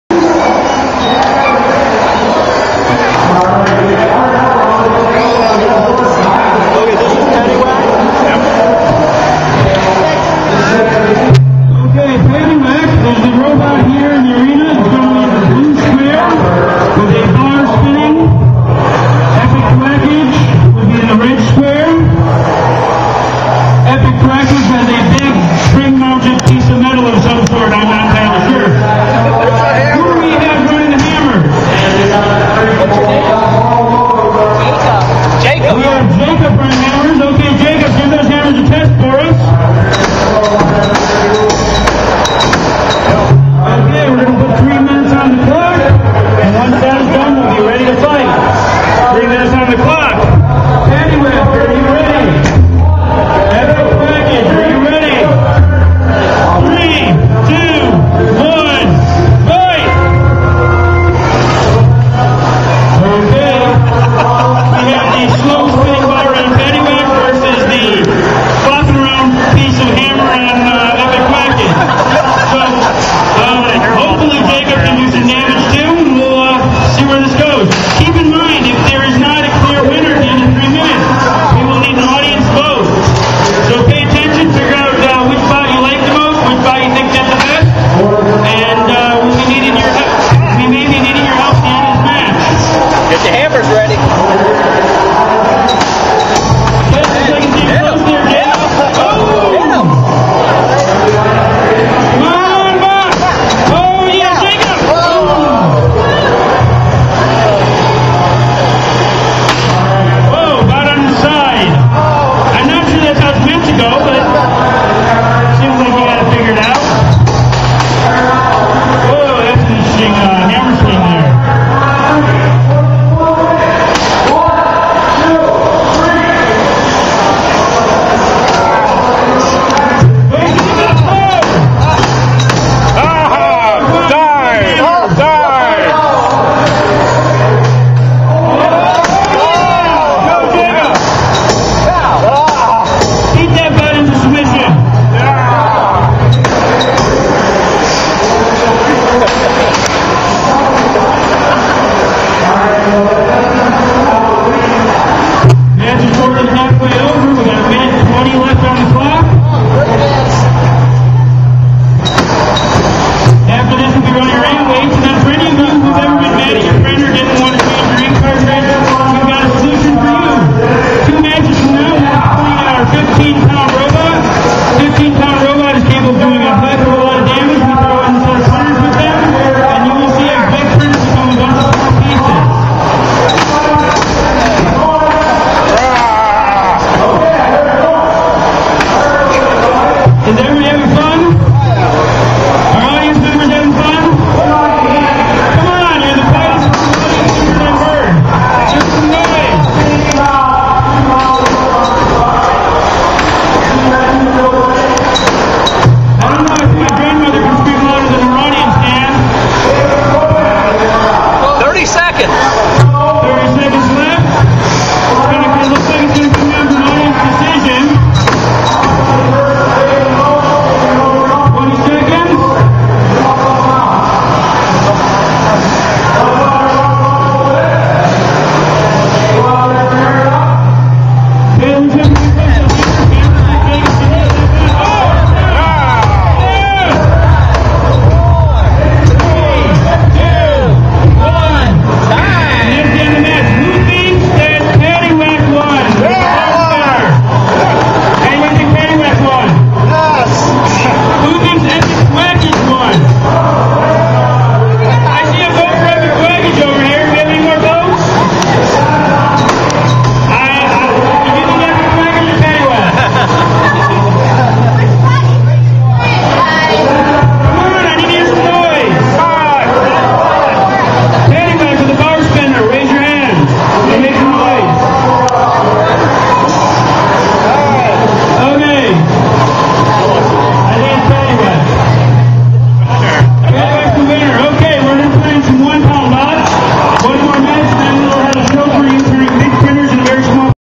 Mecha Meyhem 2010: Epic Thwackage vs. Paddywhack
Rampage Productions' Epic Thwackage versus Rampage Productions' Paddywhack at the Mecha Mayhem 2010, beetleweight class.